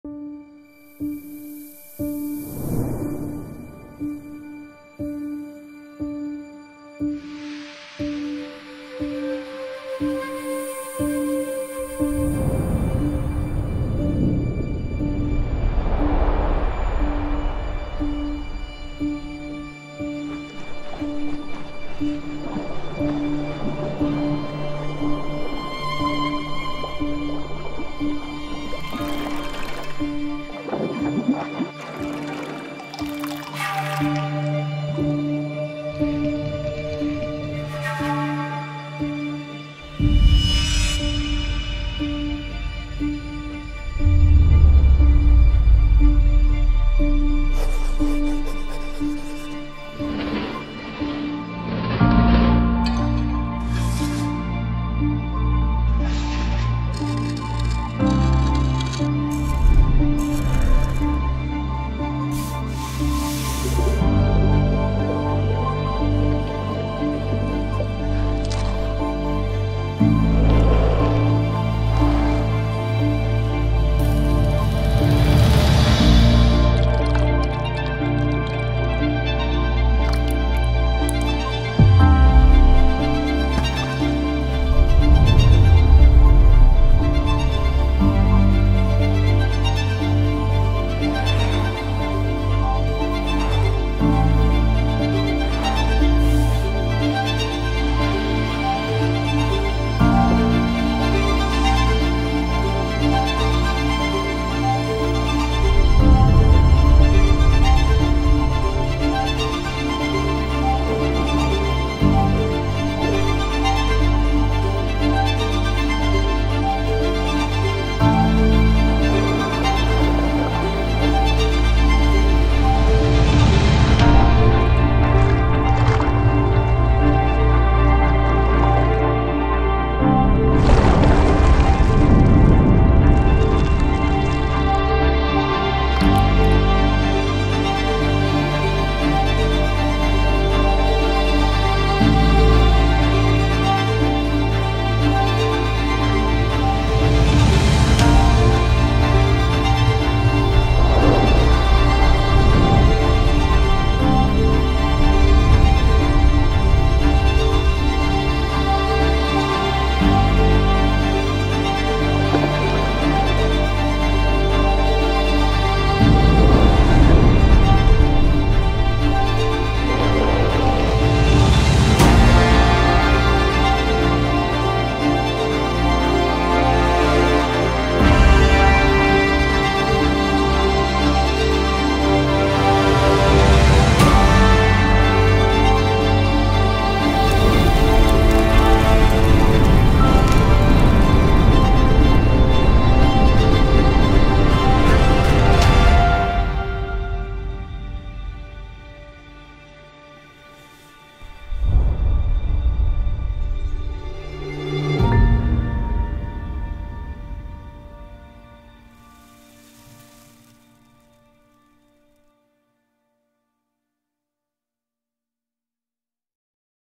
Gmundner Keramik Manufaktur - Imagefilm | Hertie
In diesem Imagefilm zeigt Gmundner ihren Prozess der Herstellung eines Kruges. Daraus wird deutlich, dass jedes Stück ein Unikat ist aufgrund dessen, dass alle Artikel per Hand gefertigt werden. Gmundner Keramik steht für Qualität und Einzelstücke.